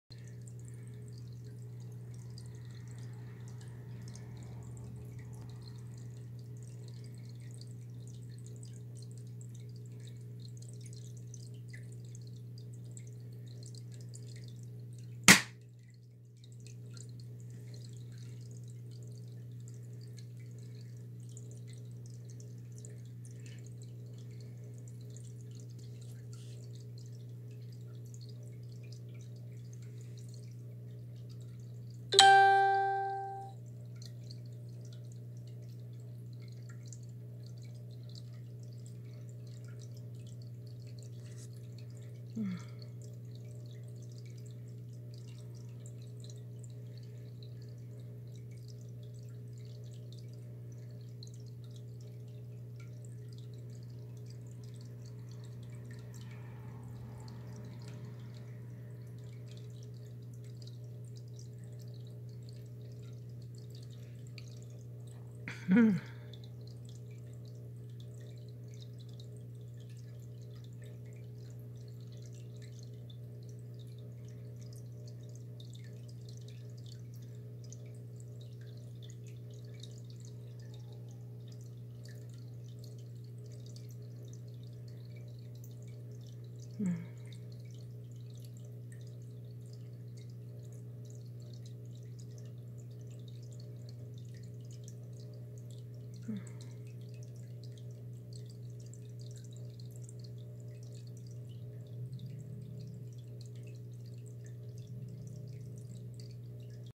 My 55 gallon studio tank filled with gold fish from David as Mother's Day surprise
The fish are enjoying their reflections!